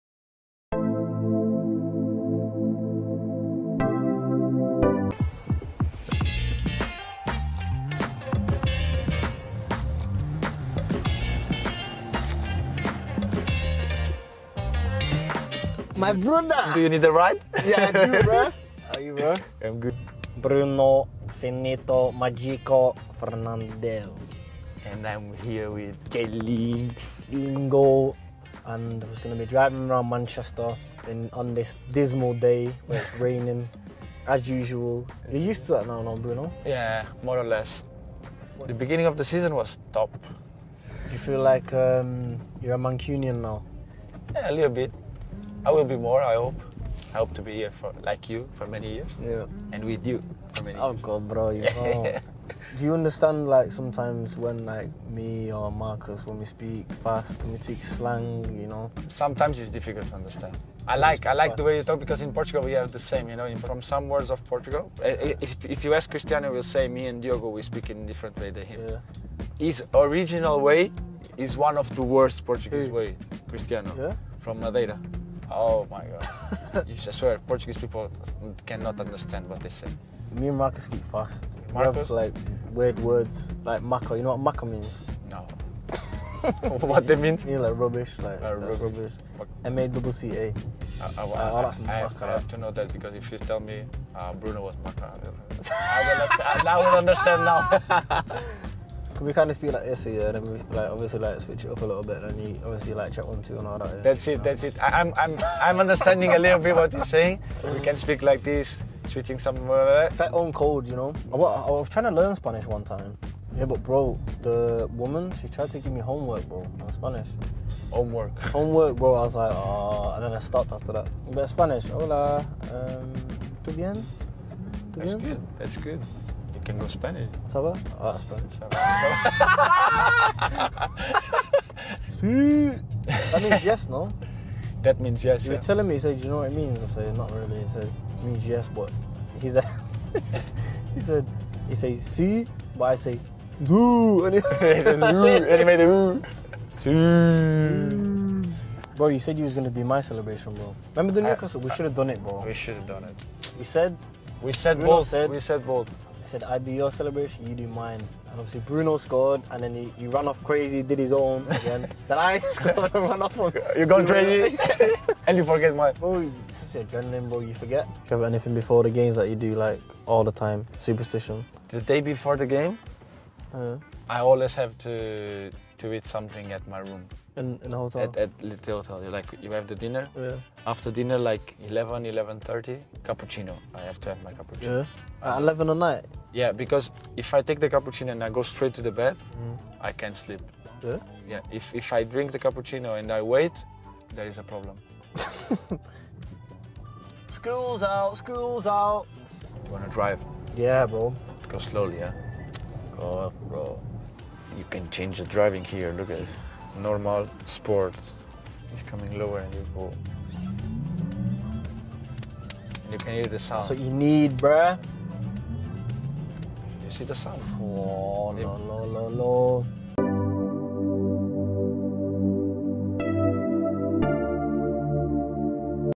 Bruno Fernandes and Lingard
Jesse Lingard and Bruno Fernandes go for a drive.